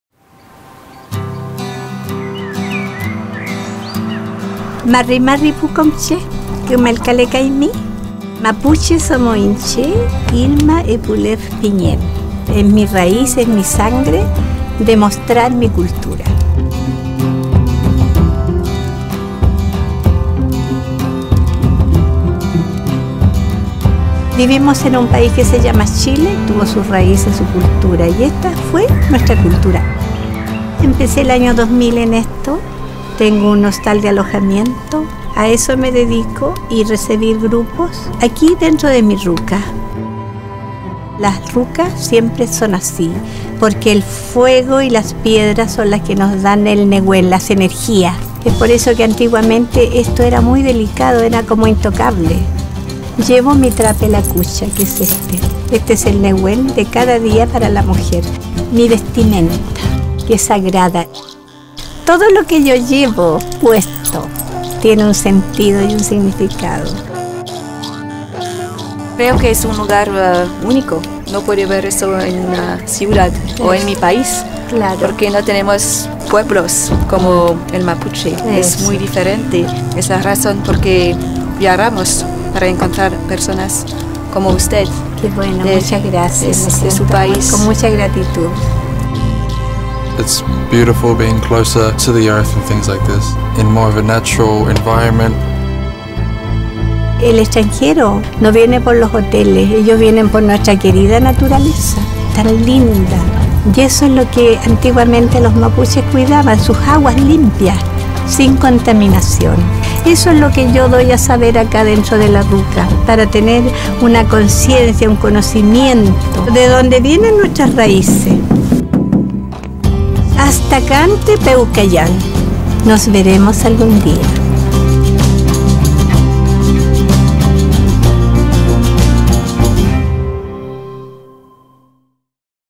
Conheça o Chile: Etnias Mapuche - Natureza Aberta
Com uma forte conexão com a natureza, o povo mapuche compartilha sua rica cultura com o mundo. Uma forma de ver o mundo onde o amor pela Terra enche cada dia de sentido e significado. Convidamos você a viver uma experiência que não terá em nenhum outro lugar do mundo.